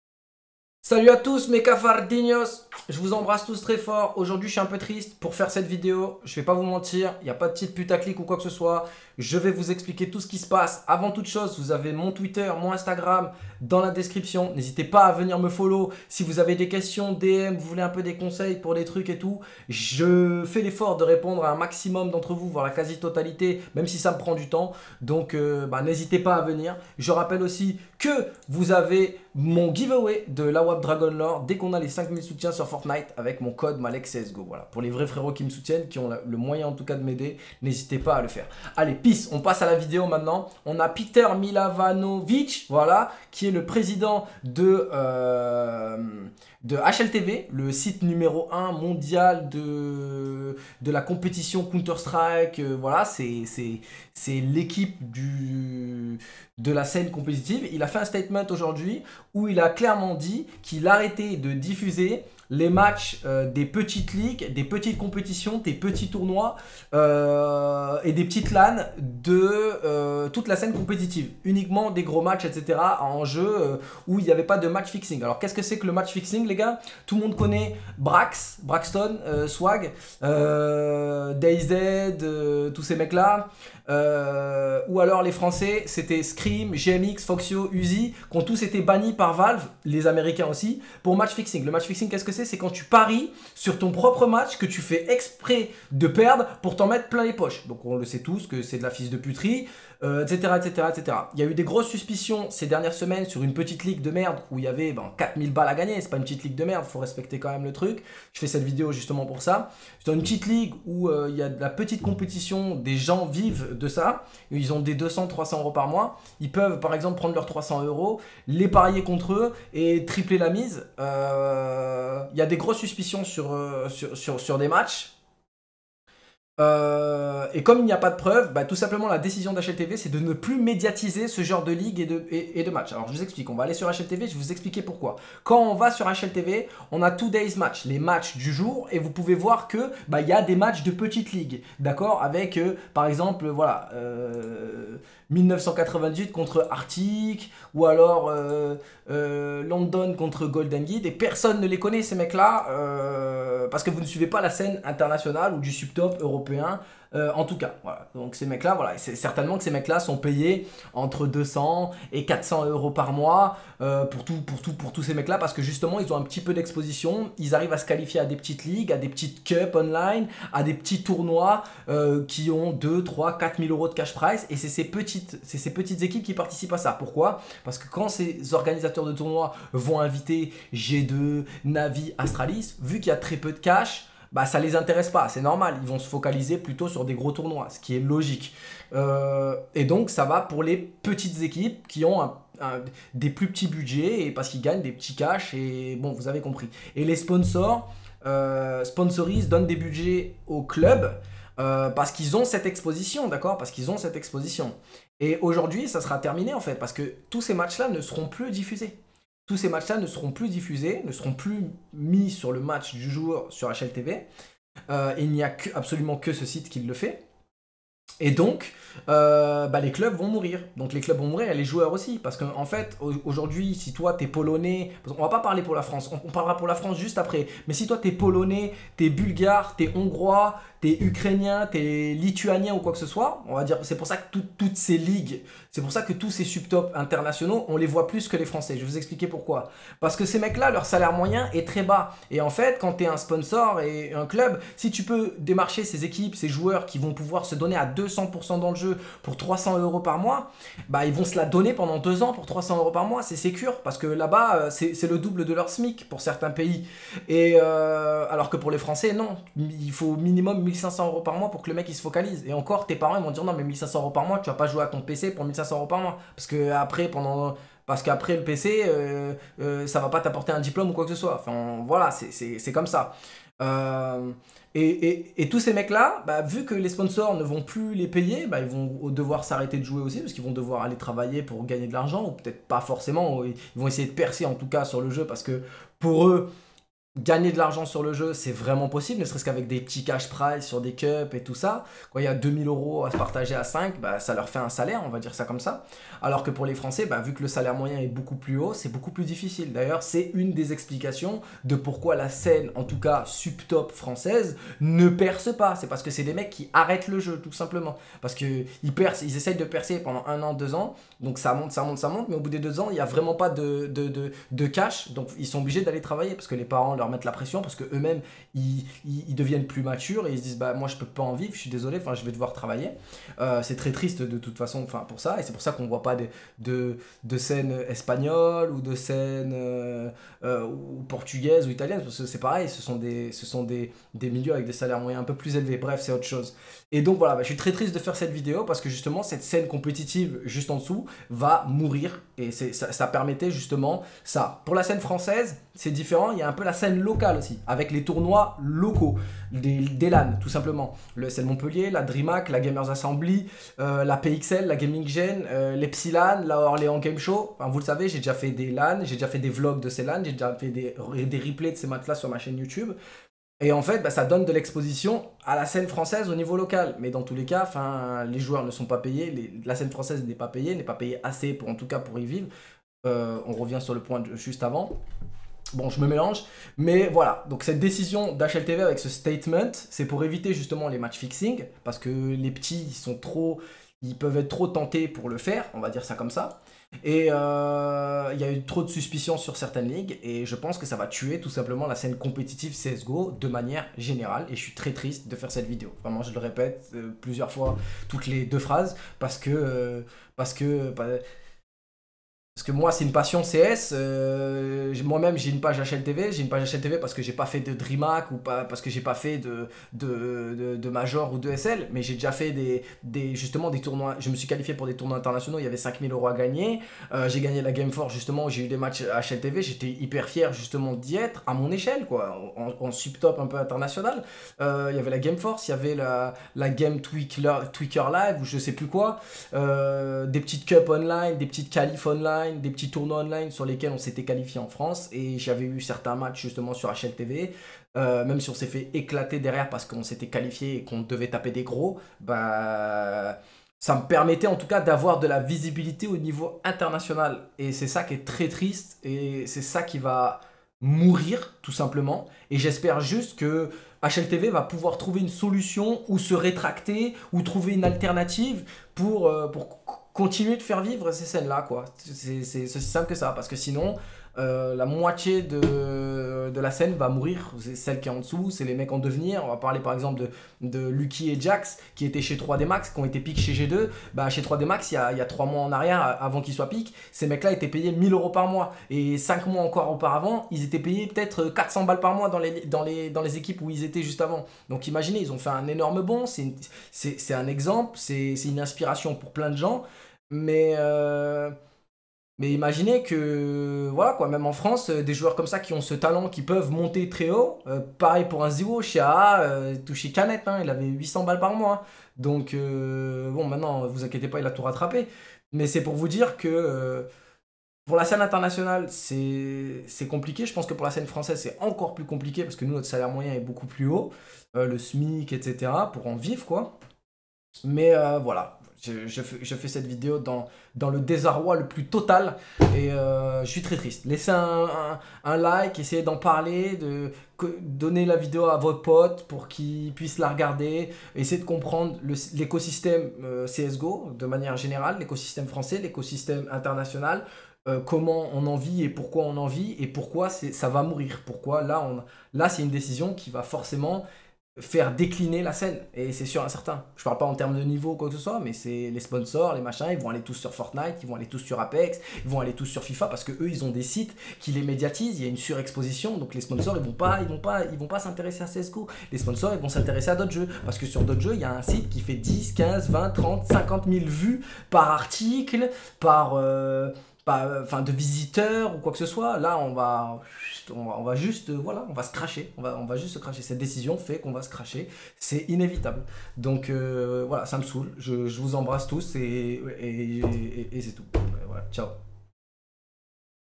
CSGO VA MOURIR LA PREUVE !!!
csgo va mourir suite a l'annonce d'HLTV c'est dramatique.
Code fortnite MALEK_CSGO
follow instagram et twitter si vous avez des questions dessous.